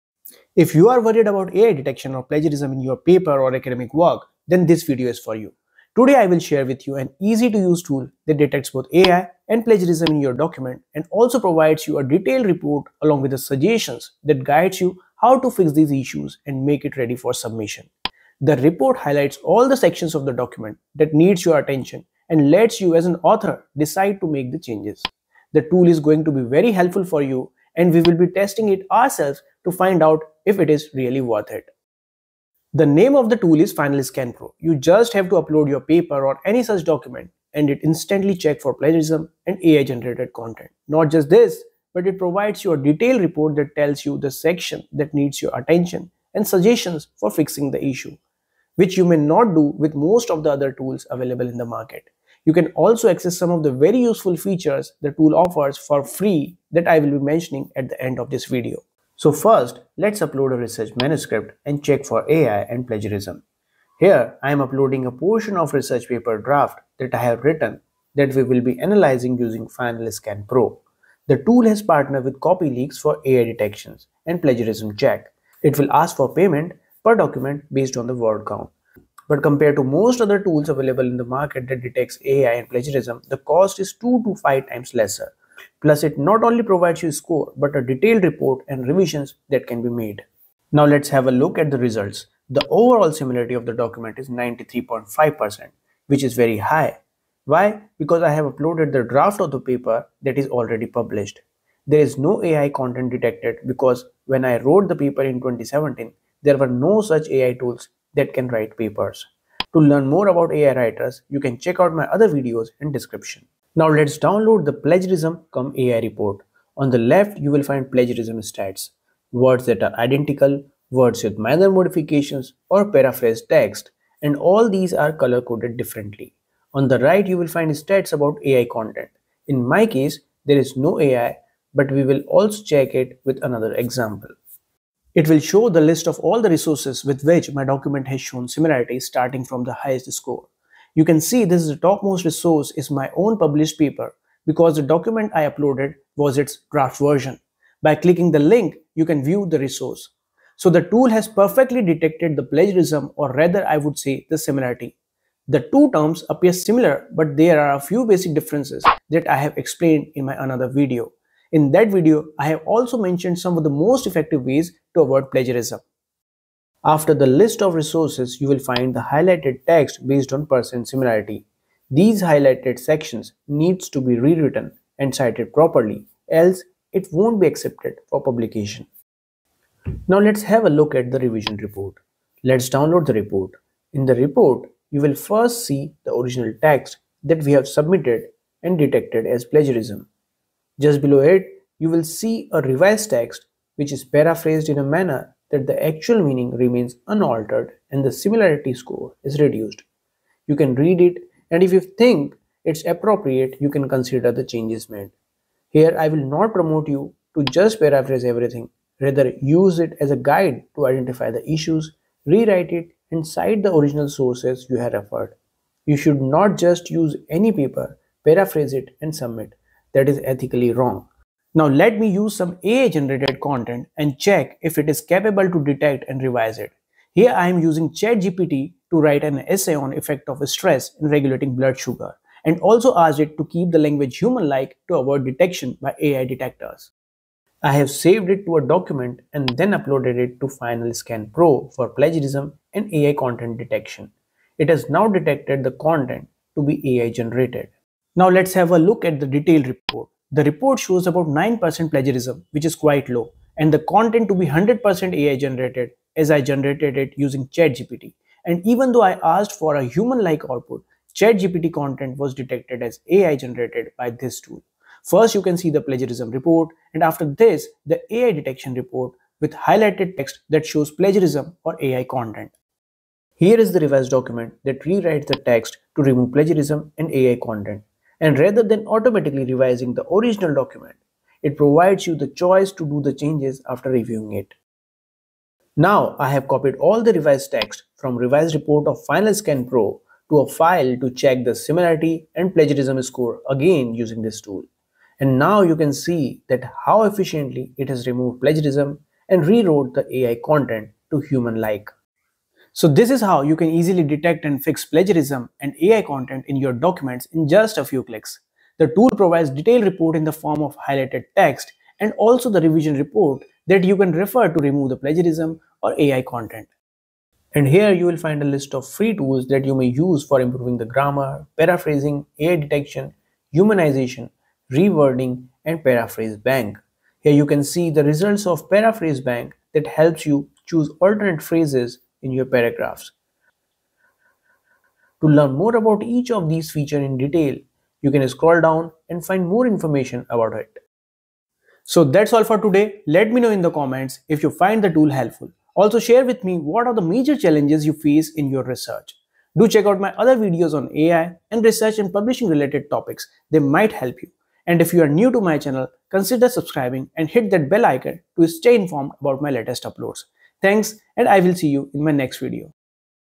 Easiest Way to Remove Plagiarism & AI Detection in Research Papers (FinalScanPro Review)
If you are looking for a single effective tool that finds plagiarism and detects AI content or a tool that helps you to instantly fix or reduce plagiarism and AI, then you should try this tool FinalScanPro. FinalScanPro helps in AI detection, plagiarism check and instantly fixing these issues. If you are into academic writing, writing research papers, reviews, writing thesis or essays then FinalScanPro can help you avoid plagiarism and AI detectors. It provides you AI and plagiarism score and detailed report for plagiarism and AI detection. This tool provides you suggestions to humanize AI written content. This tool can help you reduce the chances of manuscript rejection by reducing plagiarism and AI detection. It provides you suggestions to paraphrase or rewrite the text to make the manuscript easy to submit. It also provides a list of free tools to improve grammar, reword, paraphrase etc.